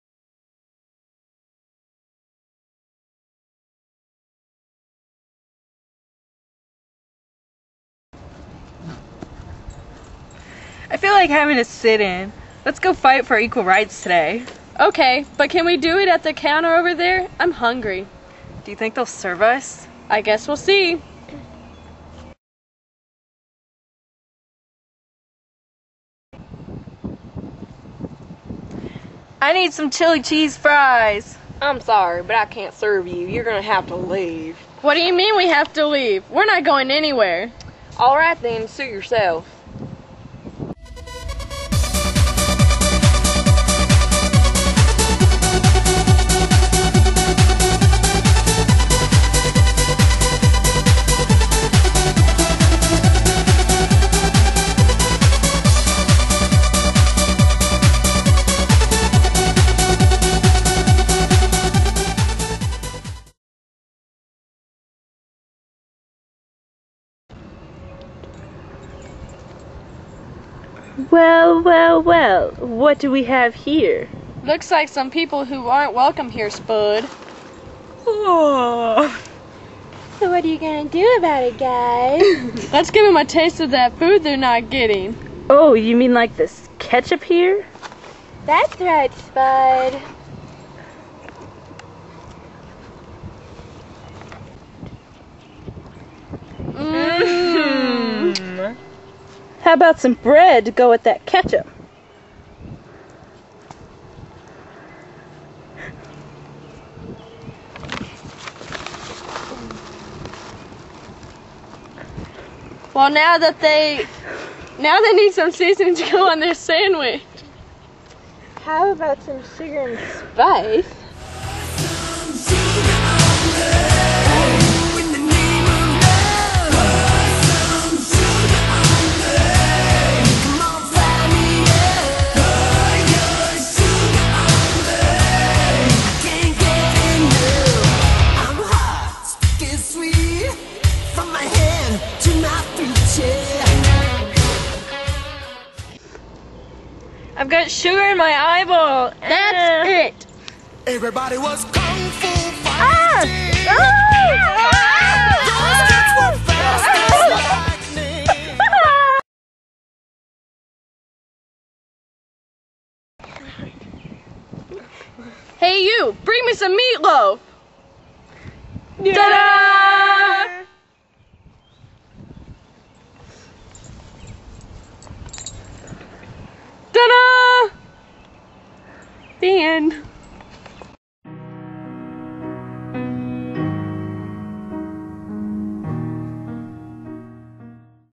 A Fight for Civil Rights
This is our American History project that we got an A on. We had way too much fun doing this. Three of my friends are five of the characters and I'm the sixth, plus the camera chick and the editor. Marvel at my talent.